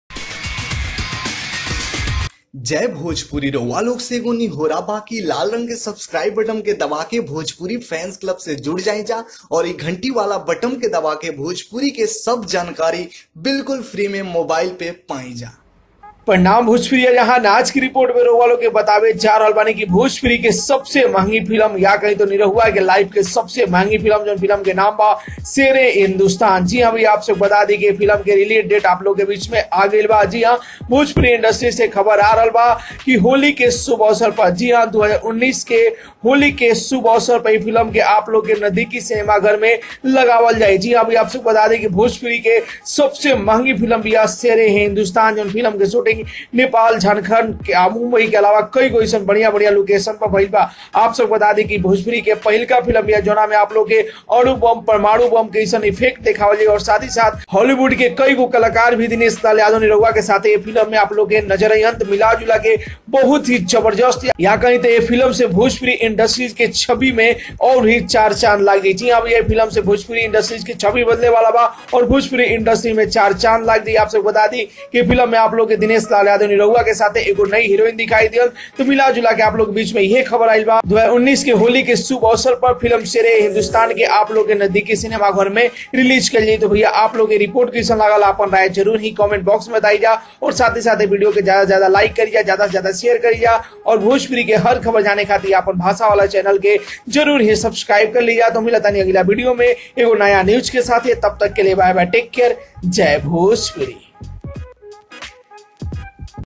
Sher-E-Hindustan (शेर ए हिंदुस्तान) - निरहुआ की नई सुपरहिट भोजपुरी फिल्म -इस दिन रिलीज होगी
Dinesh Lal Yadan "Nirahua" New Film - Sher-E-Hindustan Release Date.

Credit & Thanks : - Worldwide Record Bhojpuri, Nirahua.

sher-e-hindustan